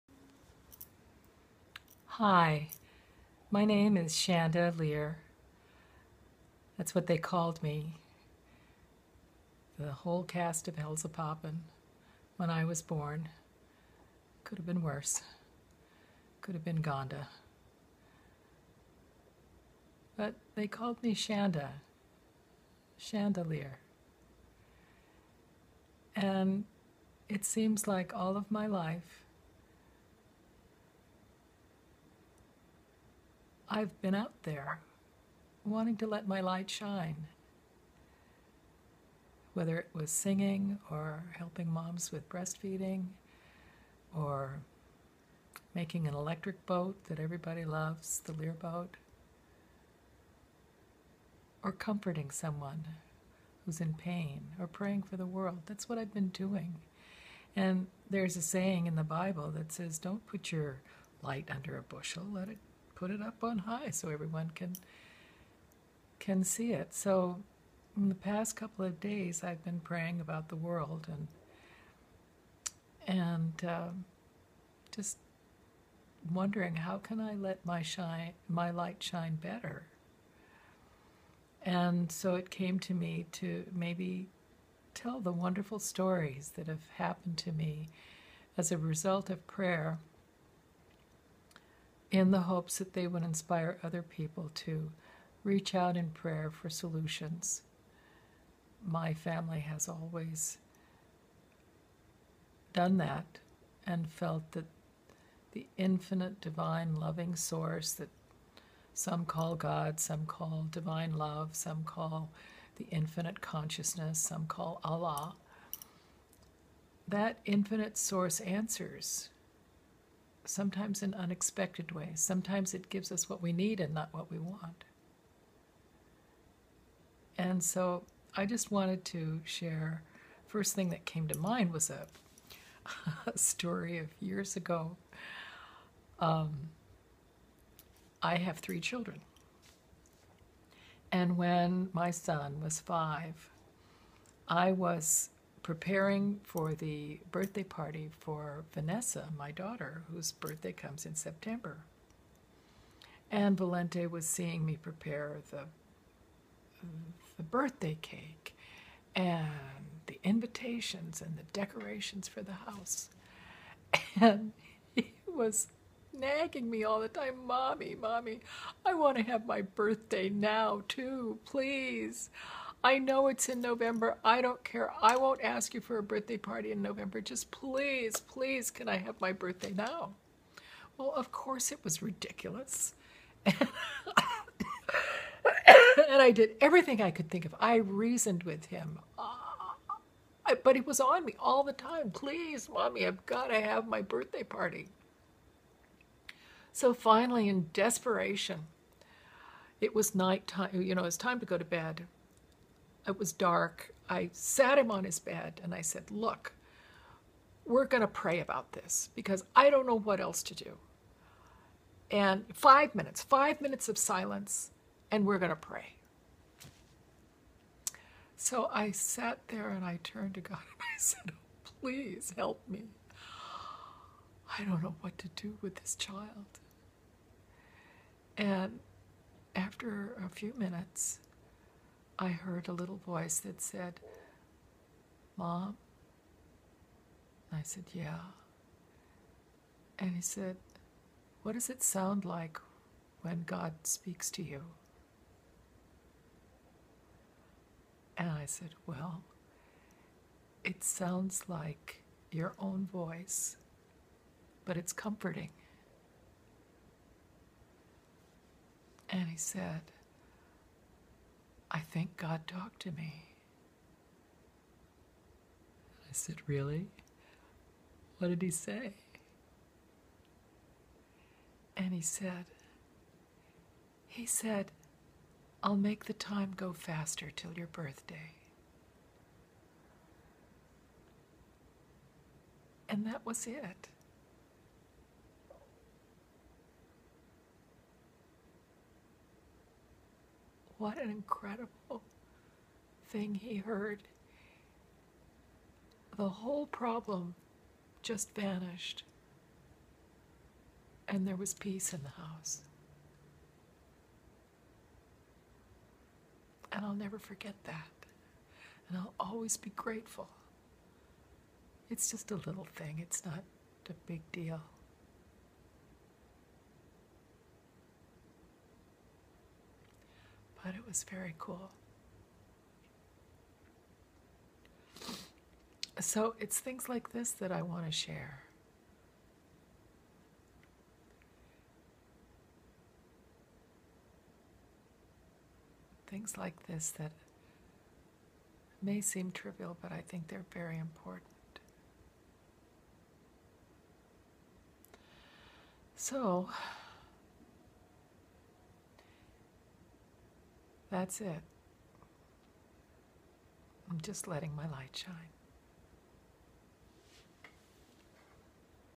1 - Introduction to Letting My Light Shine - "I'll make the time go faster 'til your Birthday"
I'm so grateful that as a little child, I learned that I could reach out in prayer for help. As I was praying for our world the other day, it was like I was being told to let my light shine and not keep it under a "bushel". "That is phrase from the Bible that I had read many times." But this time, I heard it as if it were a command. So I asked some friends what i should do. The response was to record my stories on my cell phone and then upload them to You Tube and then create a link to my facebook page. Sooo that is what I am doing. I'm just being obedient. ... and letting my light shine. .